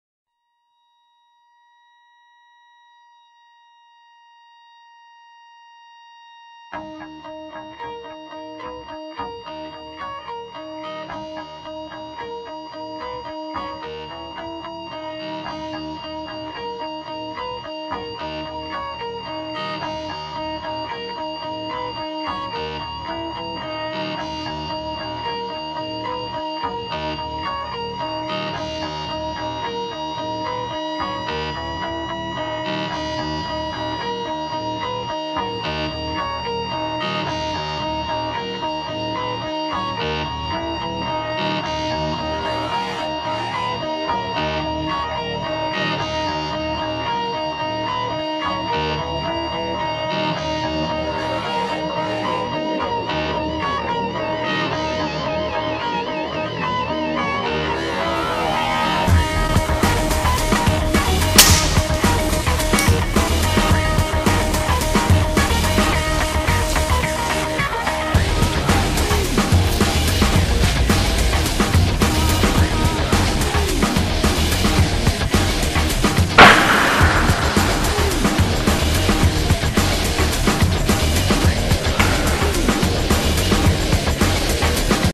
the almost impossible shot.wmv
using a hw45 .22 airpistol to pull off an almost impossible shot